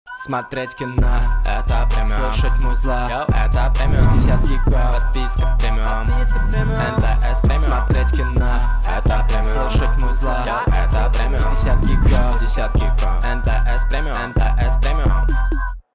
Кино на KION в подписке МТС Premium с Нагиевым (2022)
Реклама интернет-кинотеатра KION с Дмитрием Нагиевым.